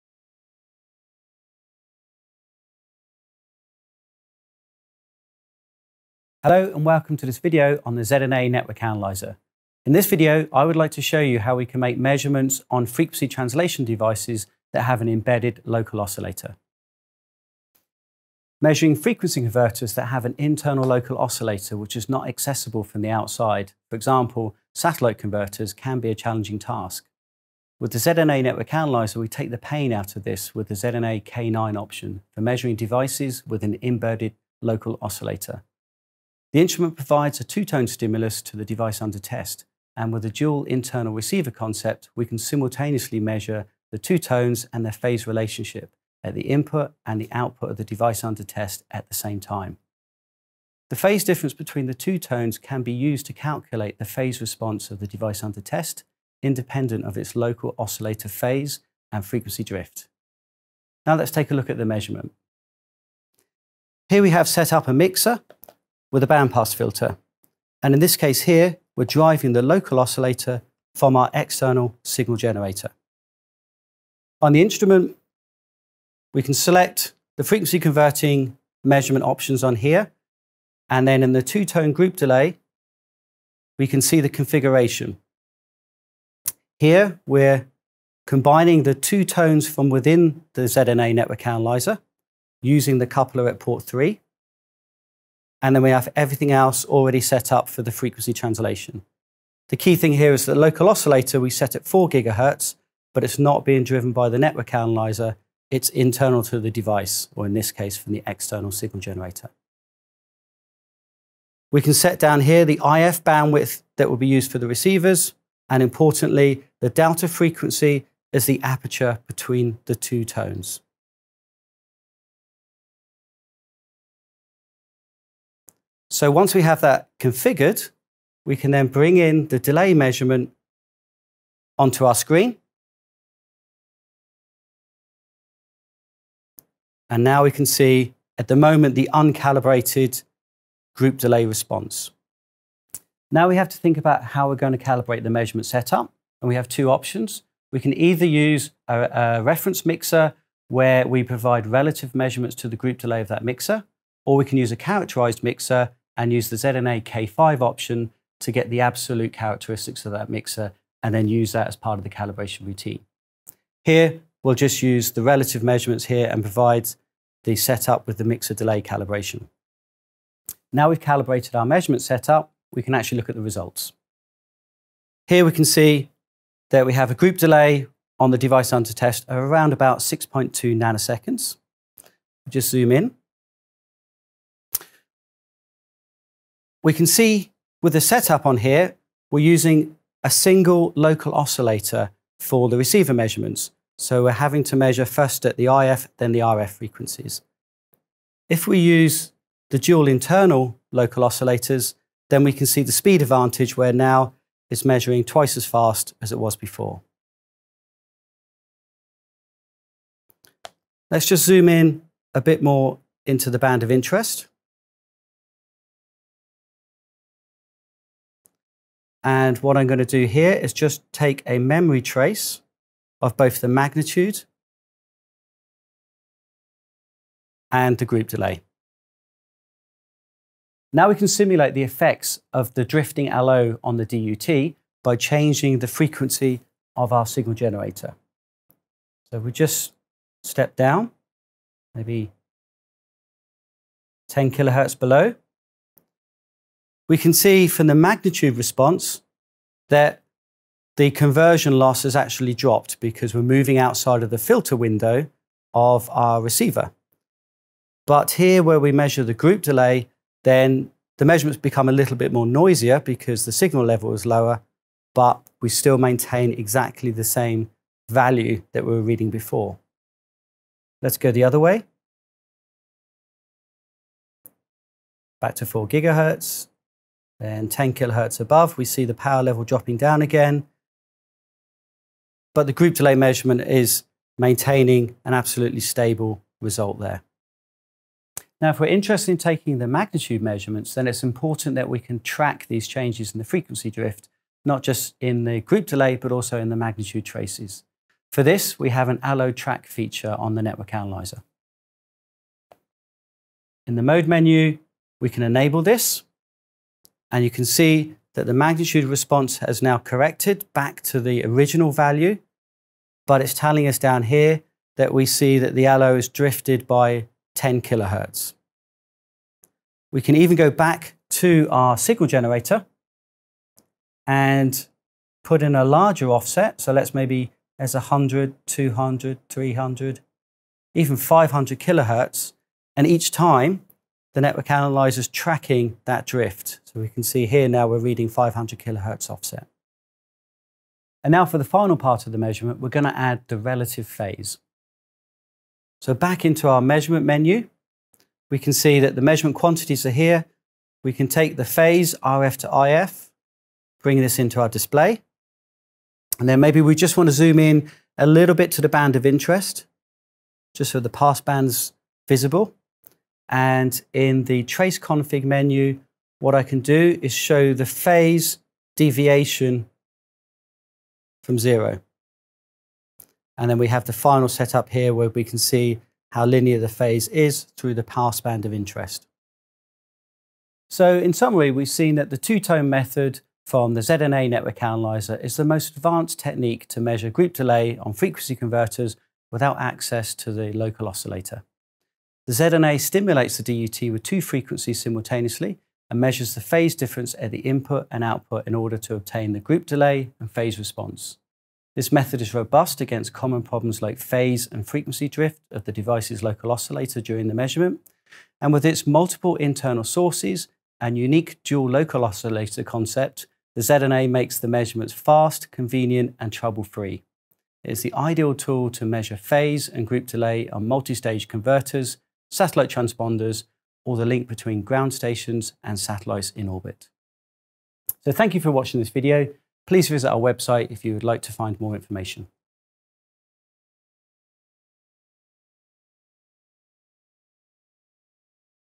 R&S ZNA Vector Network Analyzer: Group Delay Measurements on Mixers without LO access
This video presents how to perform measurements on frequency converting devices that have an embedded LO with an R&S ZNA. The advantages of the R&S ZNA solution for group delay measurements on such devices, including the LO tracking feature, and the dual LO concept are shown.